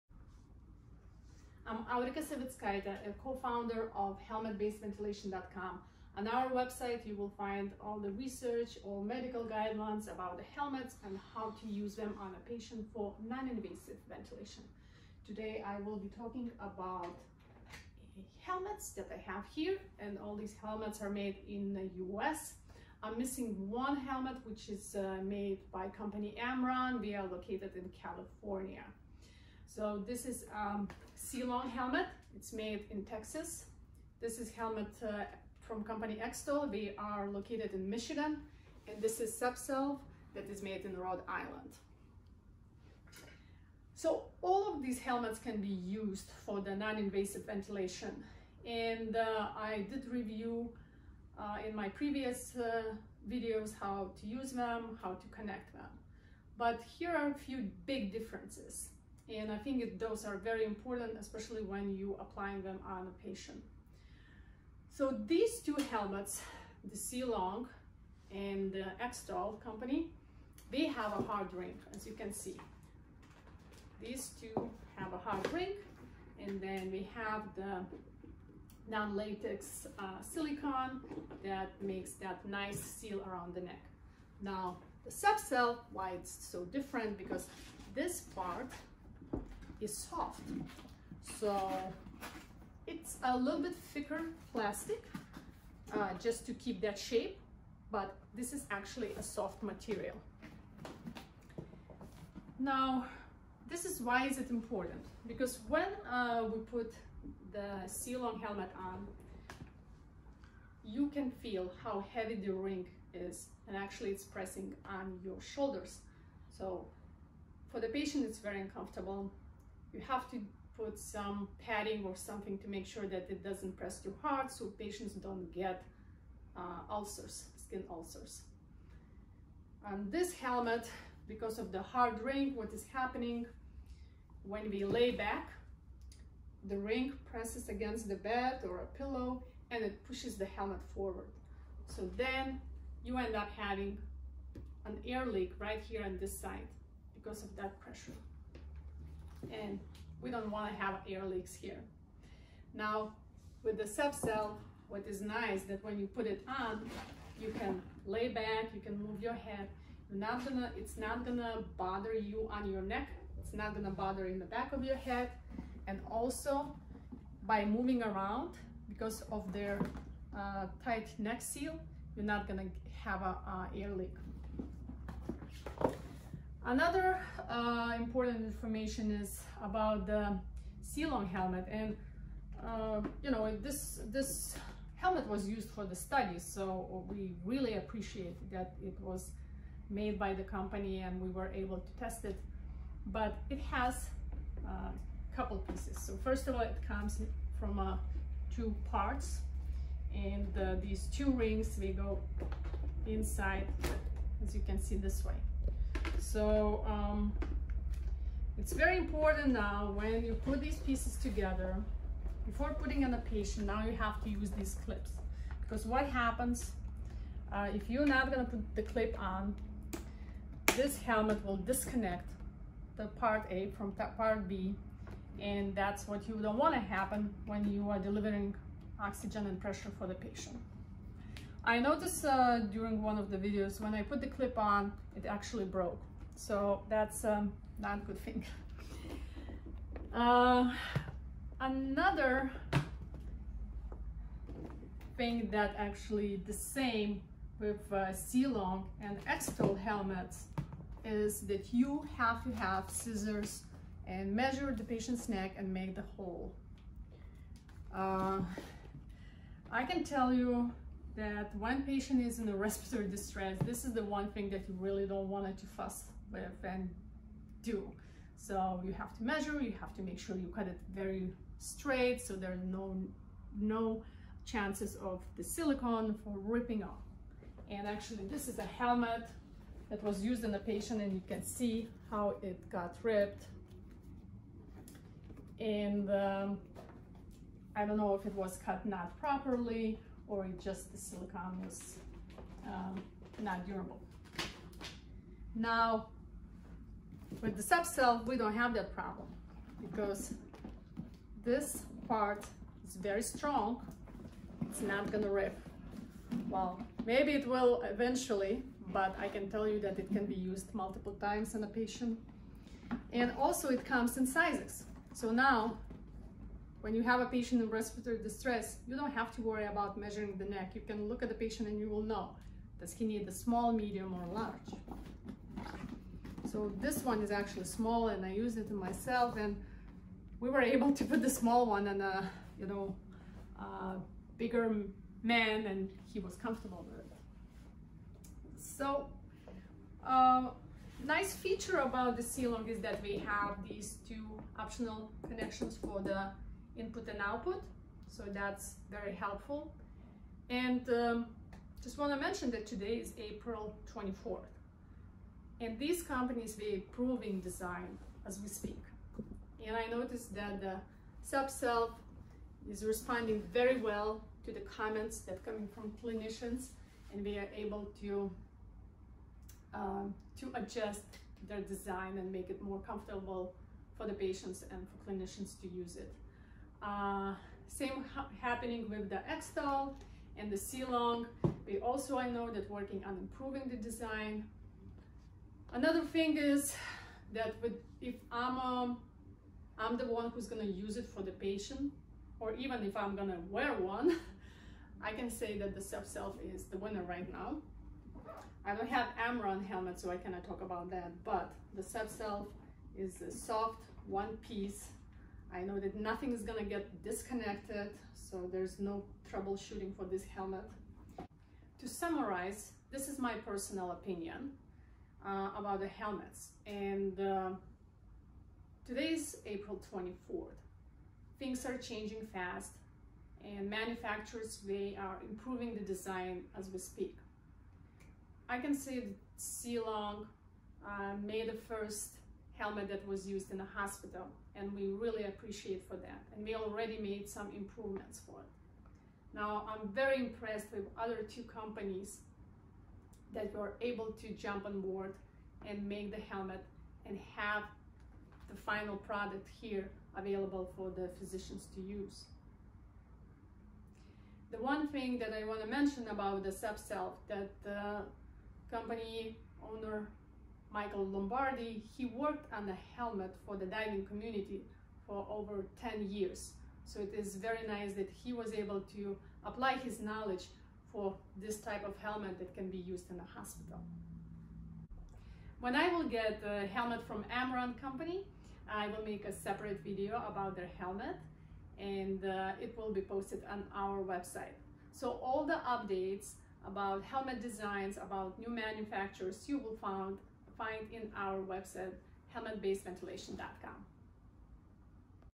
A review of 3 NIV "Bubble" Helmets made in the USA by Sea-Long, Extol & SUBSALVE / COVID-19
As of 4/24/2020, there are four NIV helmet manufacturers in the USA.

In this video, you will see 3 noninvasive ventilation helmets "oxygen hoods" made by:
Sea-Long from Texas, FDA approved.
Extol from Michigan, FDA approval is in process.
SUBSALVE from Rhode Island, FDA approval is in process.

We didn't have the 4th manufacturer's Amron from California helmet (FDA approved) at this time, we will add the link here when a video review is ready.